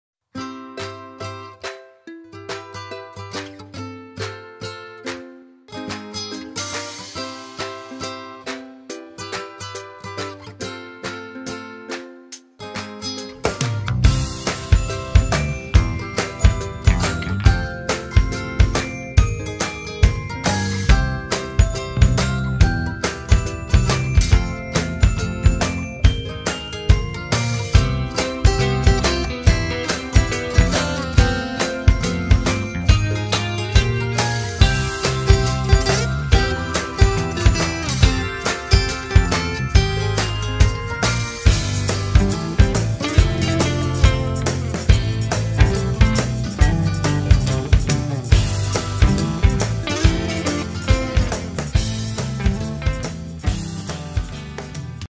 Father's Day - Bingo Industries
Entertainer. ATM. Superman. World's best babysitter. Daredevil. Role model.
Here's to you dad. Happy Father's Day from the team at Bingo Industries.